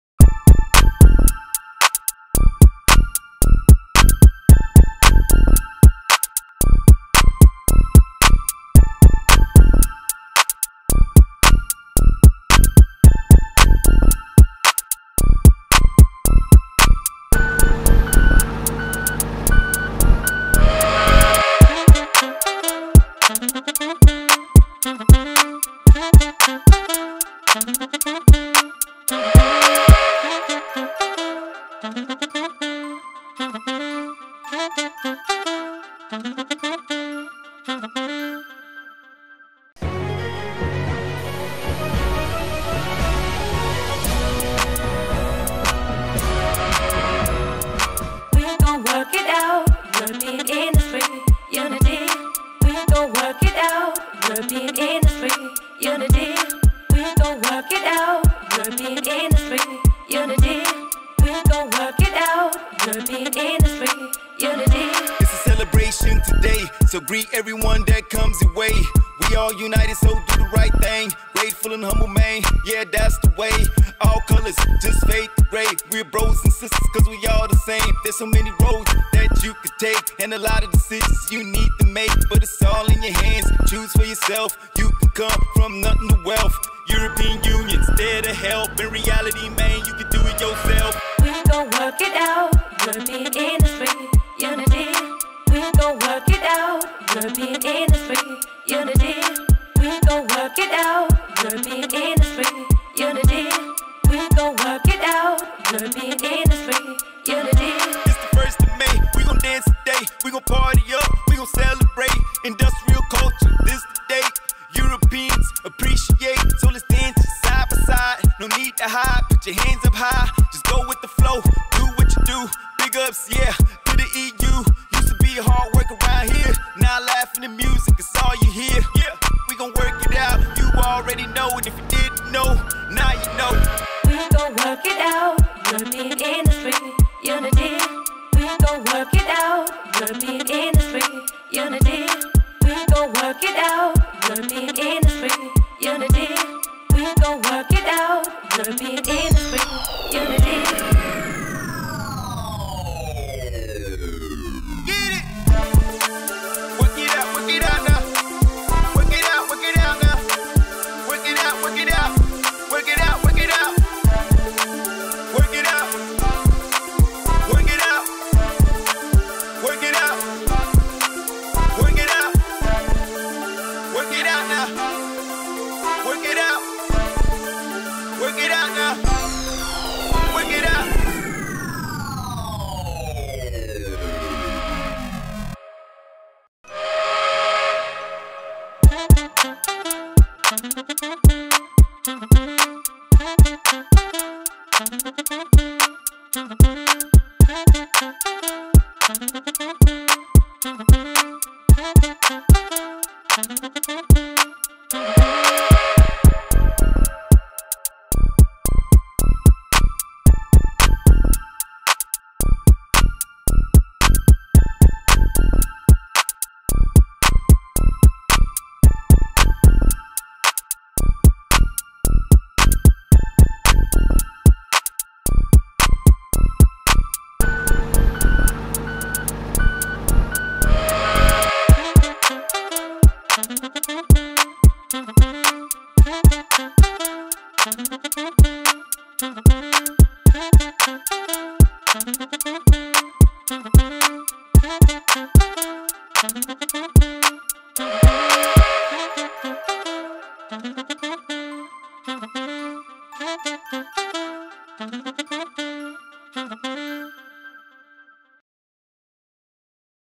ERIH WORKitOUT 2020
Mission “European Dance Event” 2020 completed: Thousands of dancers at 29 sites in 9 countries celebrated the “Day of Industrial Culture”. ERIH once again set a European example: on 13 September 2020, the "European Heritage Day | Open Monument Day".